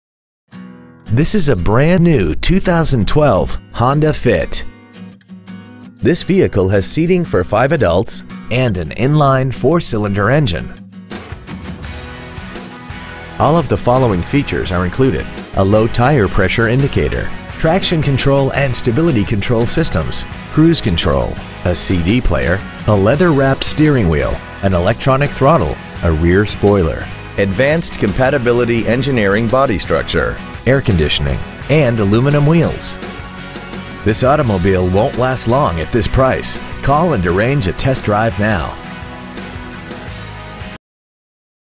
2012 Honda Fit Corpus Christi TX 78415
We are proud to present this 2012 Honda Fit .

 Year : 2012
 Make : Honda
 Model : Fit
 Engine : Gas I4 1.5L/91
 Trans . : Automatic
 Exterior : Alabaster Silver Metallic
 Miles : 10
 Interior : Black

 6702 South Padre Island Drive